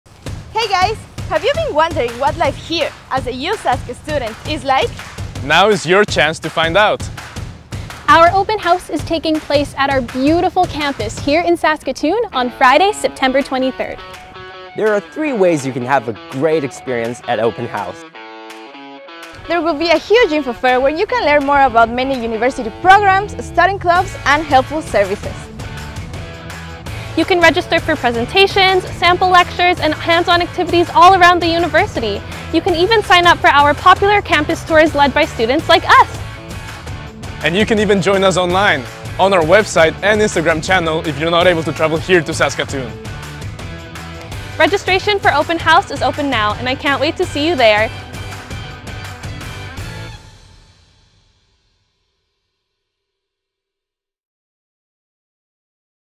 USASK Open House - Friday, September 23
USASK Open House

If you are planning for your future and wondering what life as a USask student is like, don't miss our annual Open House event, taking place on Friday, September 23!

Join us here at our beautiful main campus in Saskatoon, SK for a full day of learning opportunities, including an information fair and live presentations and demonstrations taking place across campus.

Or check out our online information sessions if you prefer to join virtually.

Open House offers something for everyone!